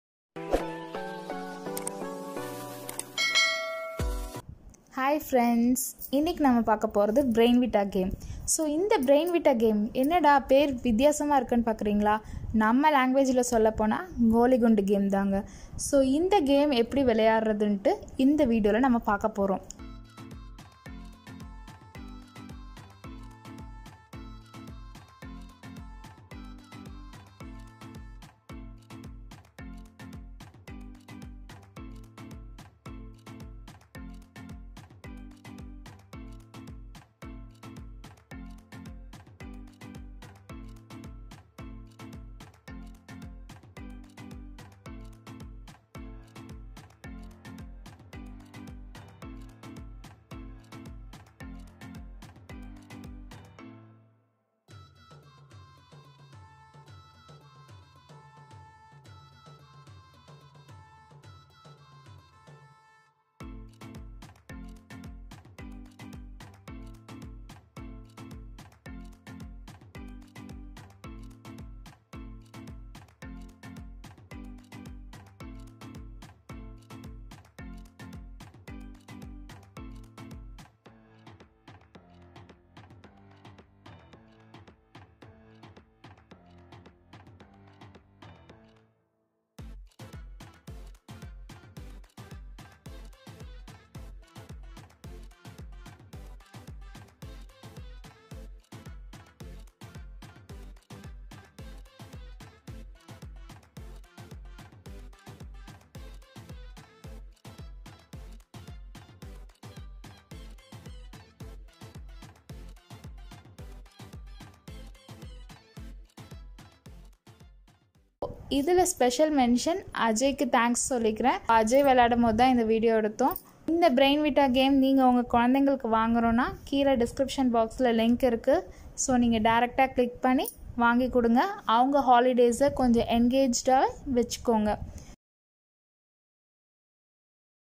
How to resolve BRAINVITA-GOLI game in tamil | Indoor game|tricks | Marble Solitaire Game |Preelasha
Hiiii friends
BRAINVITA - a board game for the brainy.

Brainvita, also called Peg Solitaire, is a single person board game. It involves moving marbles from one position to another position on the board according to rules. The objective is to have as few marbles as possible at the end of the game.
It is a classic mind game, played by a single player.
The standard game fills the entire board with marbles except for a single hole.
The objective is to empty the entire board except for a single marble.
We have provided different board layouts to test your skills.

In This Video Showing Marbles Game Easy Solution. This Board Game Called As Marbles Brainvita OR Marbles Solataire Game. Follow The Steps As Shown In Video To Solve This Game Simple And Easy. All The Best. Hope You Like This Video

Marbles Brainvita Game Rules (Rules Of Play)

1.Place 32 Marbles On The Carities Of The Plastic Tray Learning The Centre Carity Vacant.

2.Start Removing The Marbles,One at a Time. Marbles Are Removed By Jumping Over The Adjacent Marble In Horizontal or Vertical Direction. Diagonal Move Is Not Allowed.

3. The Game Is Over When There Is No Chance For Jumping Over The Adjacent Marble.

WHEN YOU FINISH YOUR GAME
If One Marble Left You Are Genious (You Won)
If Two Marbles Left You Are Above Intelligent
If Three Marbles Left You Are Intelligent
If Four Marbles Left You Need Practice
If Five OR More Marbles Left You Are Fool

do make your time useful by enjoy playing, and make your children to make thier time useful